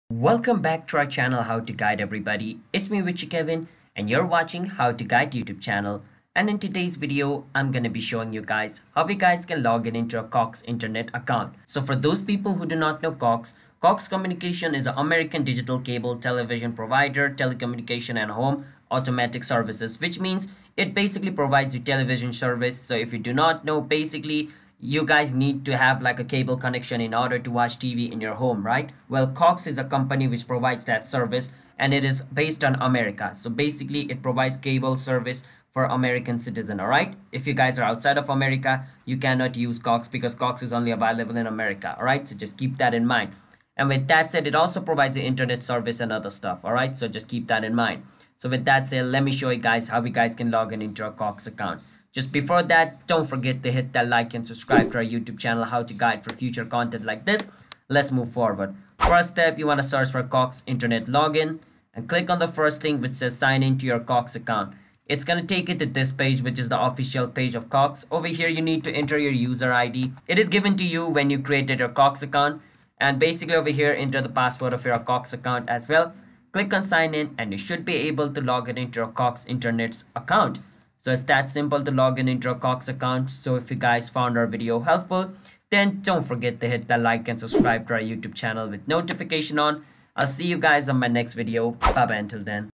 Cox Internet Login 2022 | How to Login Cox Internet Account Online
Cox Internet Login  | How to Login Cox Internet Account Online
Confused about how to login cox internet account online? This video explains the exact steps on cox internet login. Make sure you watch the video till the end to learn it in much detail.

CHAPTERS
0:00 - Intro
0:11 - How to Login Sign In Cox Internet Account Online
1:23 - Outro